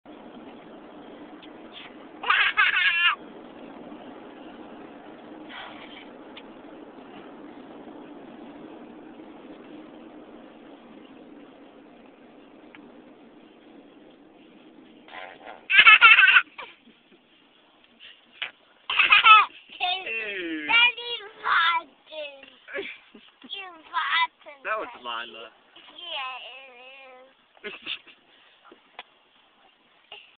Zanie...Daddy Farted!
Baby Zanie playing with the Flarp he got for christmas.  You push on it and it make fart sounds.  He thinks its hilarious!